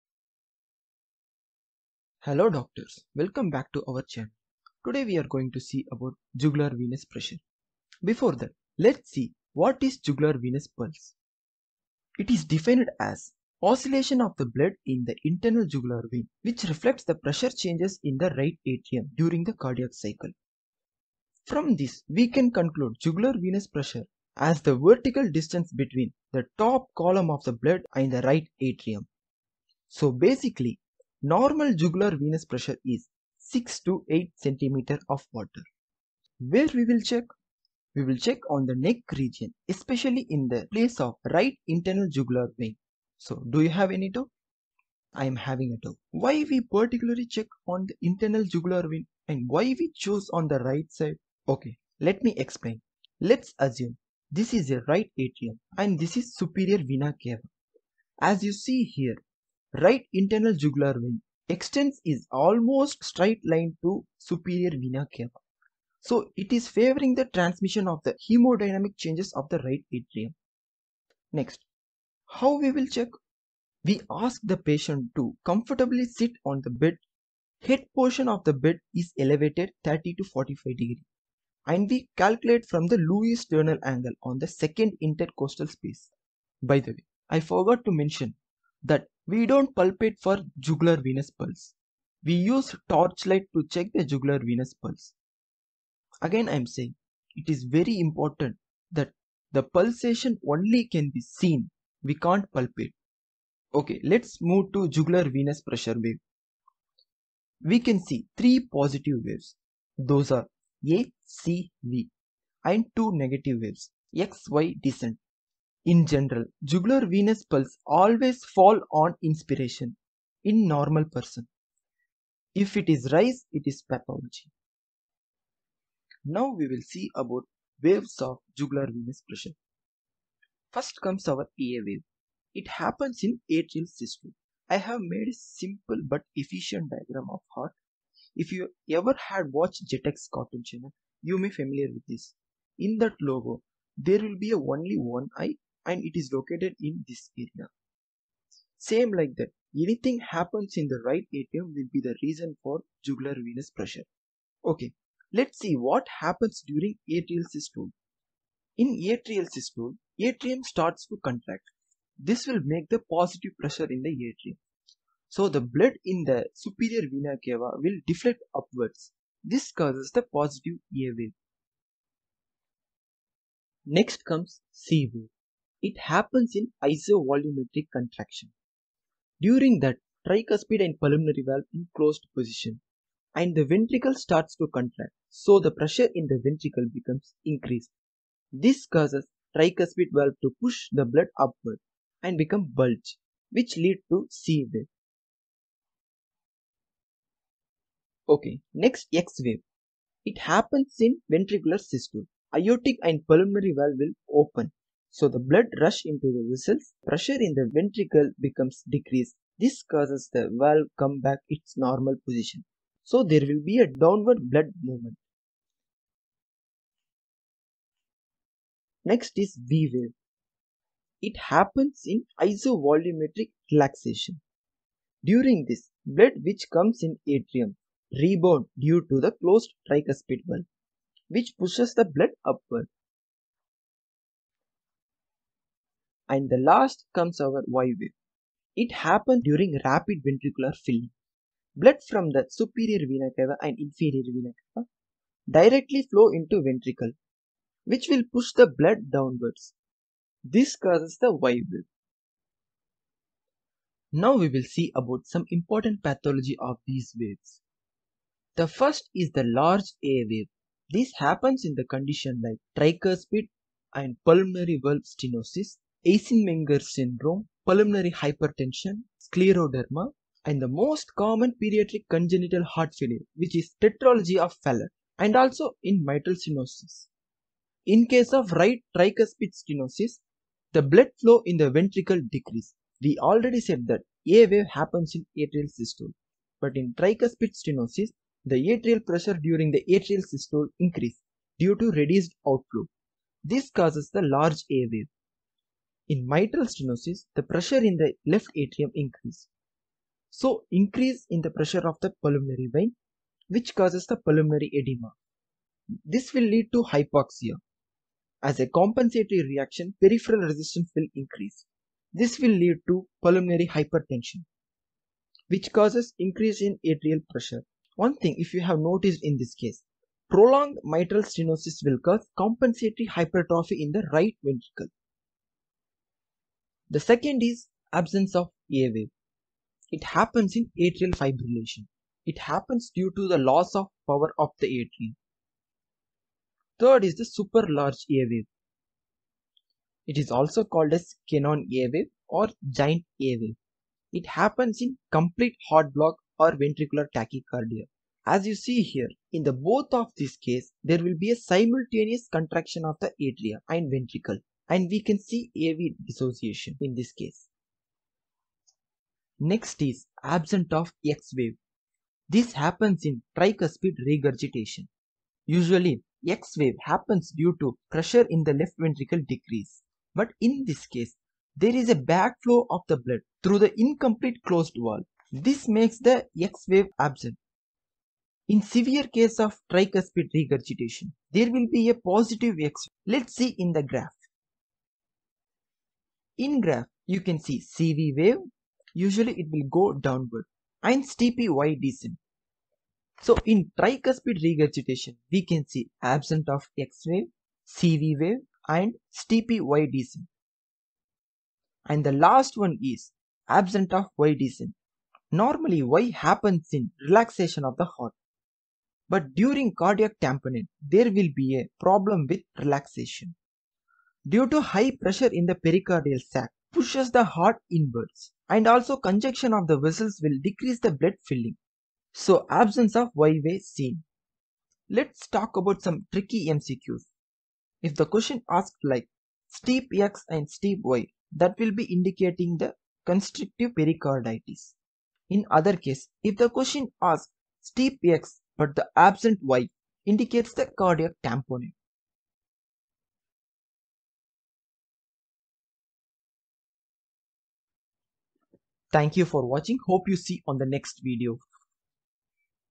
Jugular Venous Pressure- Basics,normal and pathological waves of JVP. focused on FMGE/NEXT & NEET-PG
JUGULAR VENOUS PRESSURE:
Basics.
normal waves (a,c,v,x,y) explanation on cardiac cycle

Important Pathological JVP waves:
1. Large a wave
2. Absent a wave
3. Super large/CANON a wave
4. Absent x descent
5. Absent y descent